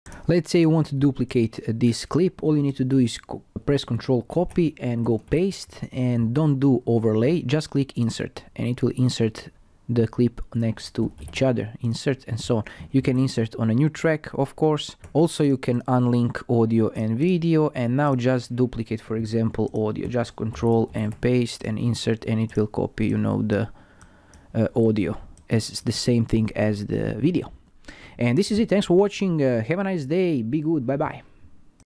Hitfilm Express Tutorial: How To Duplicate Clip In Hitfilm Express
Learn How To Duplicate Clip In Hitfilm Express in this video tutorial.